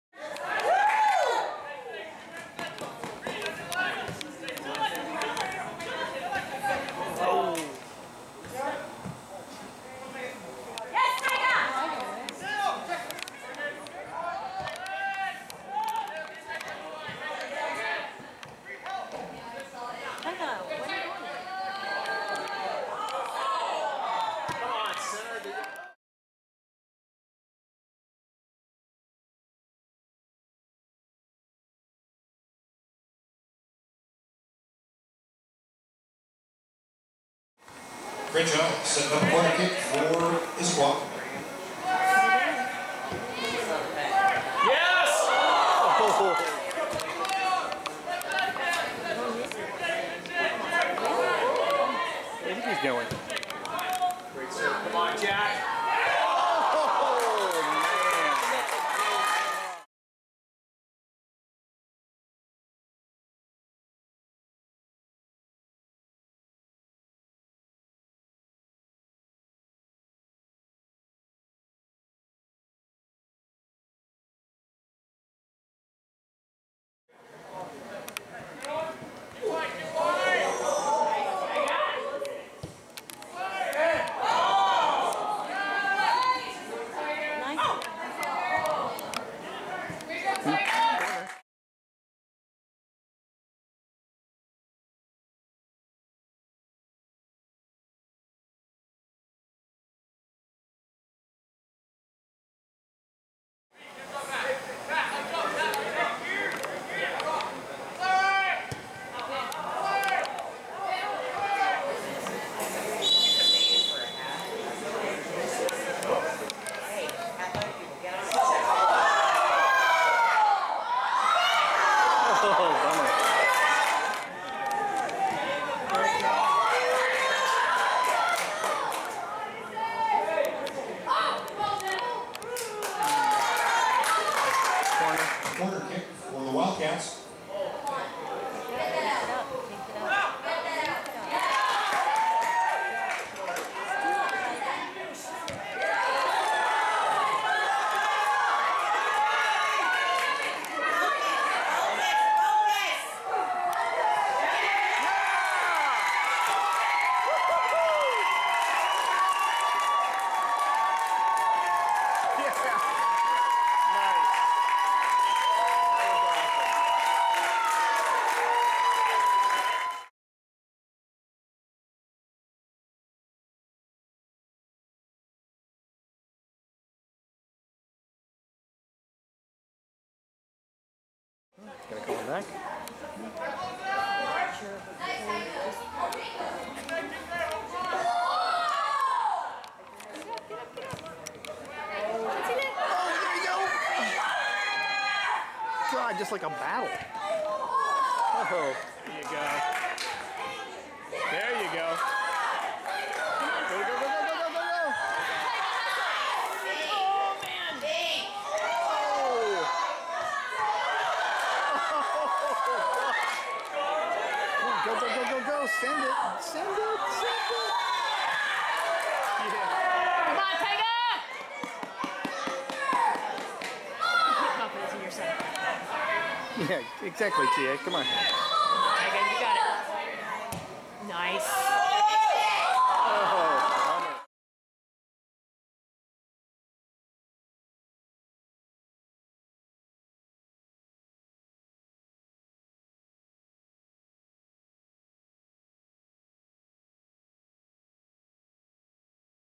Taiga (#57) assist & highlights from IHS vs. Mt Si April 11, 2017
Highlight footage of crosses, corner kicks, free kicks and an assist by Issaquah high school sophomore Taiga Crenshaw (#57, left wing) to Joey Underwood from the lHS vs Mt. Si Washington state 4A Boys Varsity game on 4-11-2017